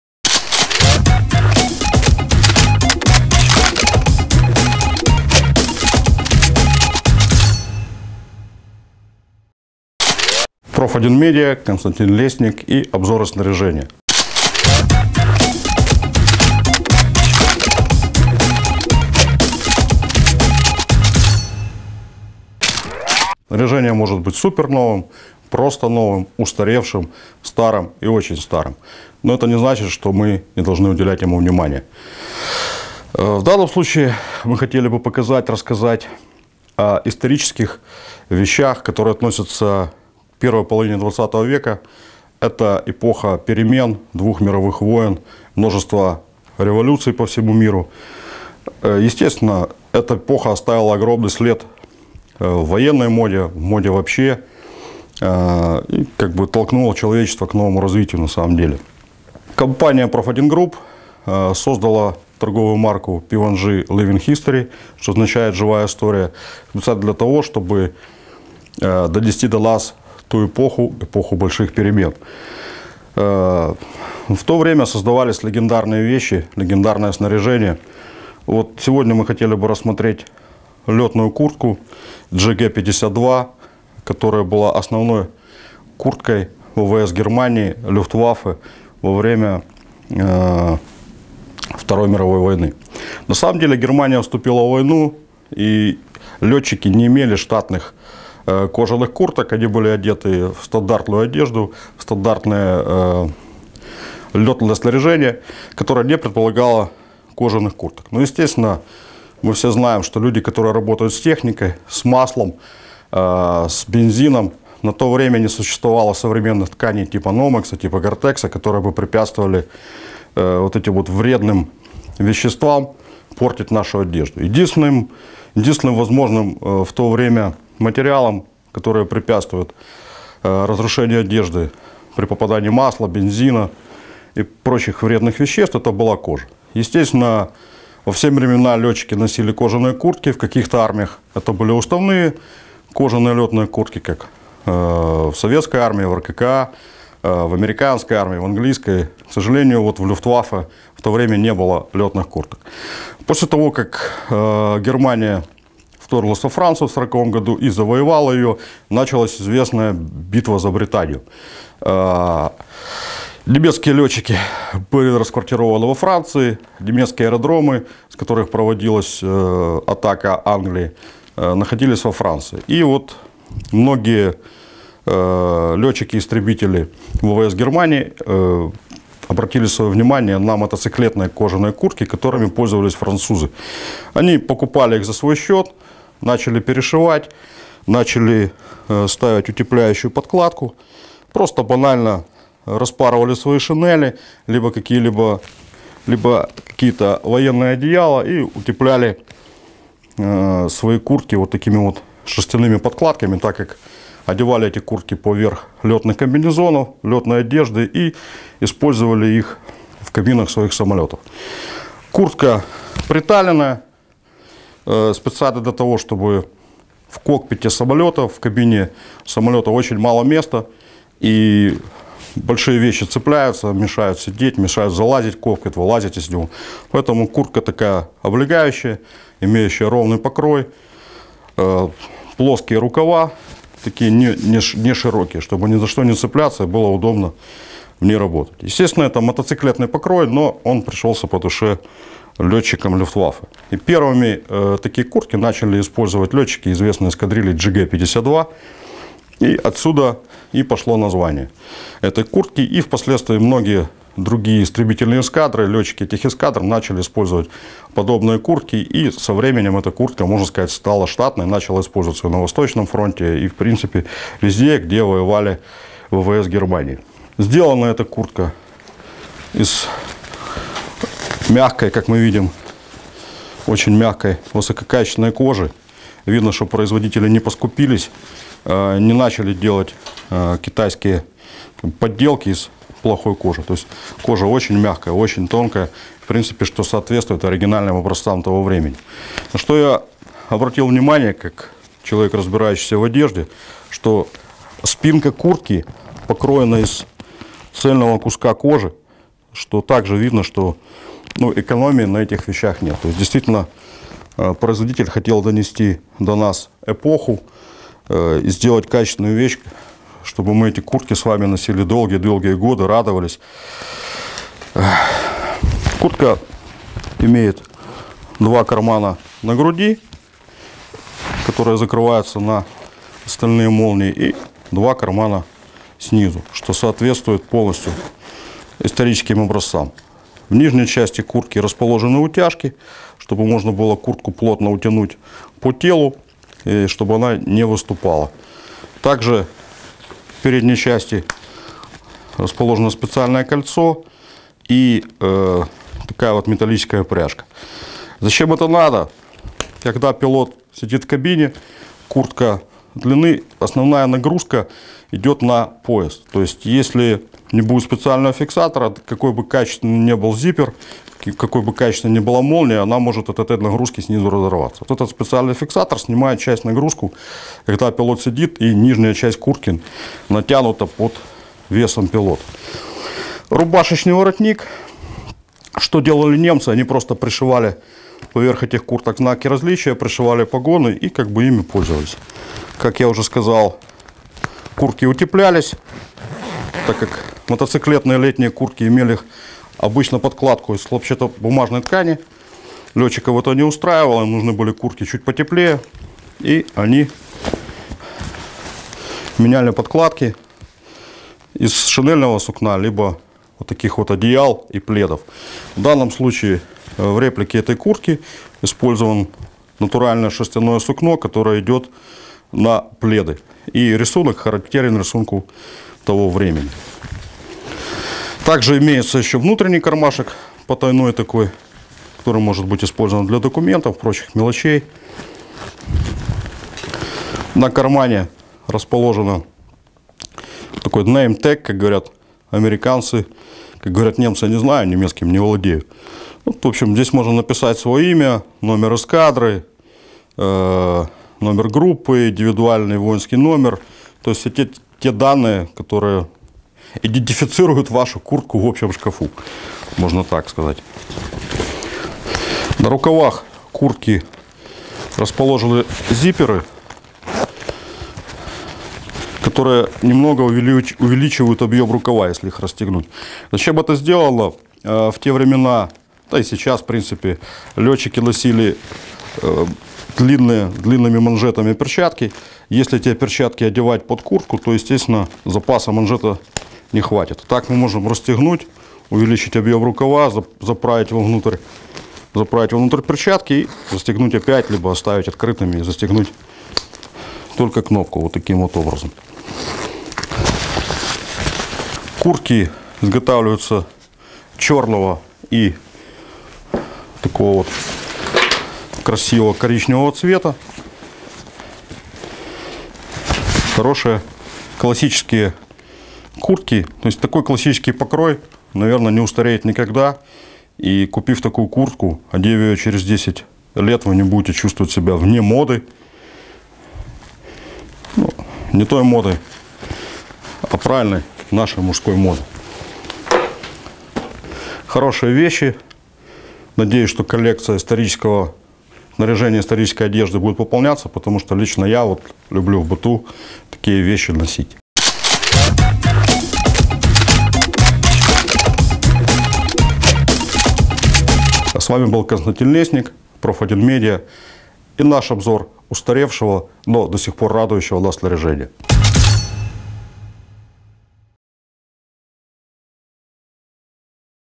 Обзор. Куртка кожаная лётная "JG-52 " 1940 Jahr от P1G-Living History
Легендарная летная кожаная куртка летчиков-истребителей ВВС Германии периода начала Второй Мировой Войны. Впервые кожаные куртки "JG-52 " были использованы пилотами 52-й истребительной эскадры Люфтваффе в Битве за Британию в 1940 году. По одной исторической версии данные куртки были переделаны из легких кожаных французских мотоциклетных курток. На них меняли подкладку на более теплую (из шерстяного сукна шинелей, военных одеял или гражданских шерстяных пледов) и навешивали знаки различия. Удобные легкие и прочные куртки пришлись по вкусу летчикам-истребителям и наземному обслуживающему персоналу ВВС и их начали использоваться практически во всех авиационных соединениях. Куртки "JG-52 " оставались в ходу на всем протяжении Второй Мировой Войны.

Торговая марка P1G Living History производит легендарные куртки "JG-52 " по лекалам периода 40-х годов 20-го века и с ипользованием высококачественных натуральных материалов. Кожаная летная куртка "JG-52 " 1940 Jahr изготовлена из натуральной лошадиной кожи ручной выделки, подкладка, как и в 40-х годах, изготовлена из натурального сукна. Также используются высококачественная стальная фурнитура и застежки "Молния" производителя YKK. Два верхних и два нижних прорезных наружных кармана с застежкой "Молния" YKK. Внутренний карман с левой стороны куртки также имеет застежку "Молния" YKK. Основная фронтальная застежка "Молния" YKK также снабжена дополнительными застежками с клапанами для усиления мест наибольшего напряжения при двигательных нагрузках. Внизу по бокам куртки расположены металлические пряжки с кожаными ремнями для регулировки объема поясничной области. В нижней части рукавом располагаются молнии и регулируемые манжеты для удобства одевания перчаток. Рубашечный воротник куртки может быть использован ,как для ношения знаков различия, так и для защиты шеи от ветра и холода.